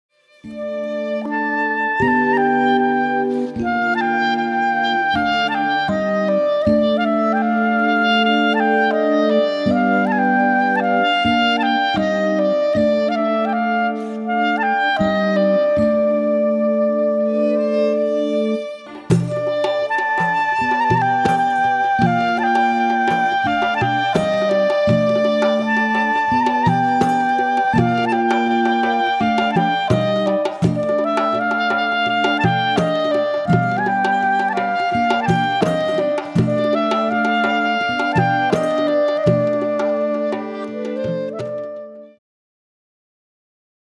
5 главных слов. Побережье - ялы бою
Ялыбой уже готов встречать гостей, любящих Крым, его природу и прекрасный морской воздух. Давайте прогуляемся по крымскому побережью вместе с подборкой из 5 слов!